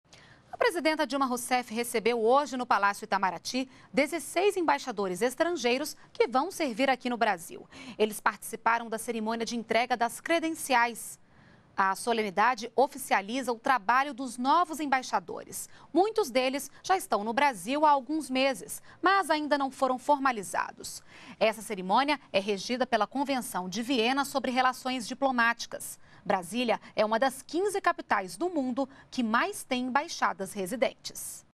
Dilma recebe 16 novos embaixadores no Palácio do Itamaraty
NBR NOTÍCIAS - 23.01.13: A presidenta Dilma Rousseff recebeu nesta, quarta-feira (23), no Palácio do Itamaraty, 16 embaixadores estrangeiros que vão, a partir de agora, servir no Brasil. Eles participaram da cerimônia de entrega das credenciais, que oficializou o trabalho dos novos embaixadores. A entrega das credenciais é regida pela Convenção de Viena sobre Relações Diplomáticas. Brasília é uma das 15 capitais do mundo que mais tem embaixadas residentes.